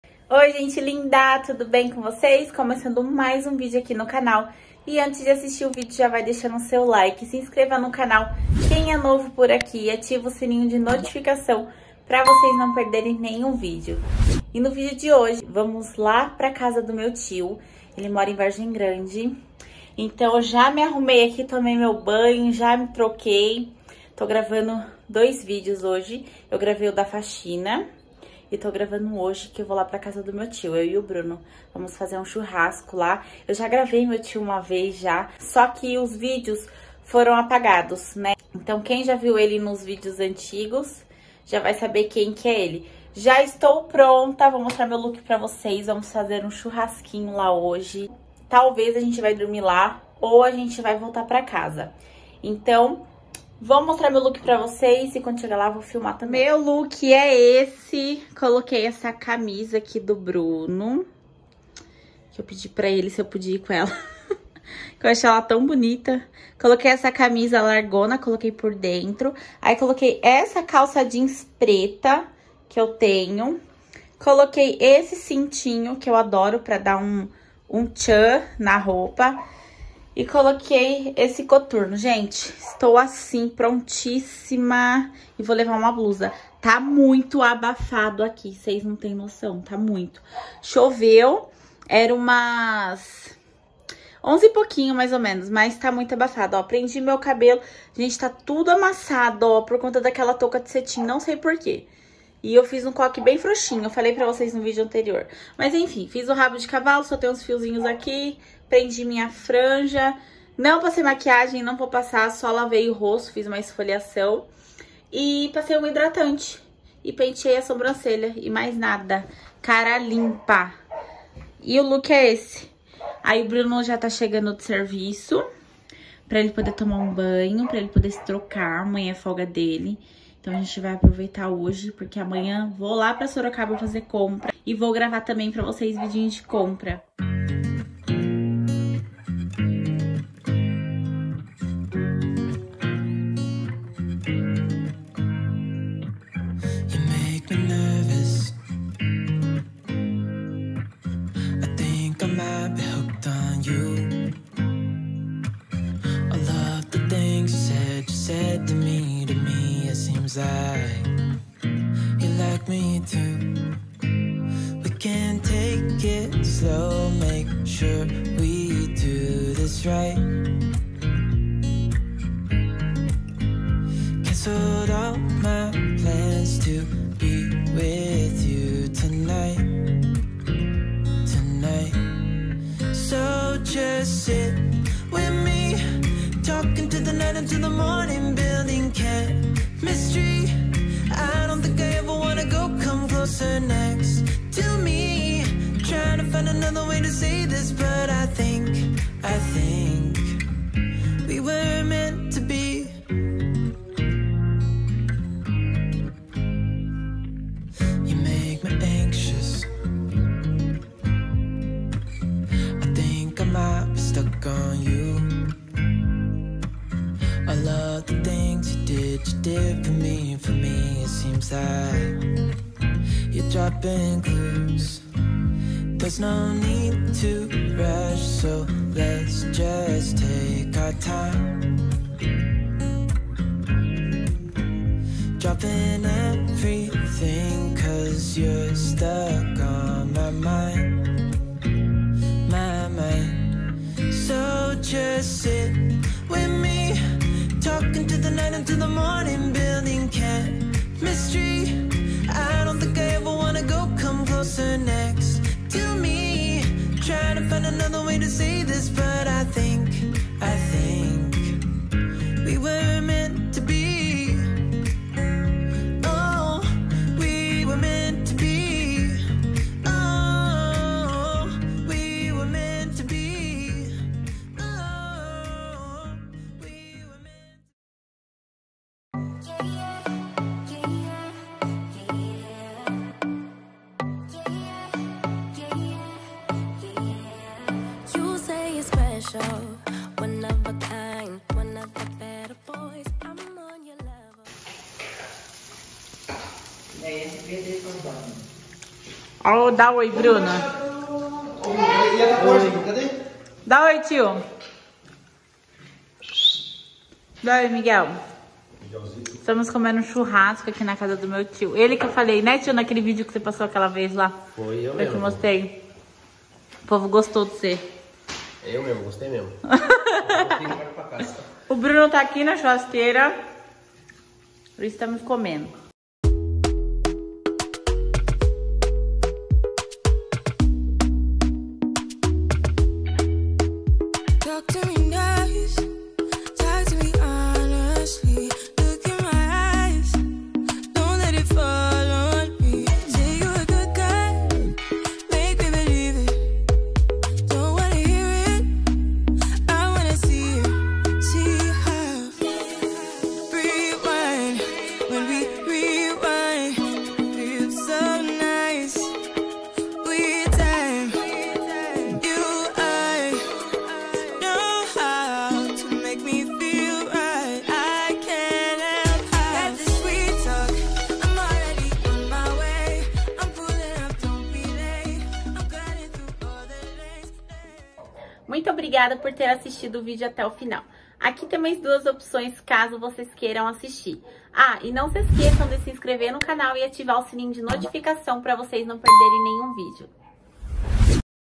NOITE DO CHURRASCO NA CASA DO MEU TIO
OII GENTE LINDA,TUDO BEM COM VOCÊS?

NO VÍDEO DE HOJE FOMOS NA CASA DO MEU TIO FAZER UM CHURRASCO A NOITE E ESTAVA TUDO GOSTOSO,ESPERO QUE GOSTEM❤️

❤️PARA QUE O YOUTUBE ENTENDA QUE VOCÊS QUEREM RECEBER OS MEUS VÍDEOS

•COMENTA💬
•COMPARTILHA🌏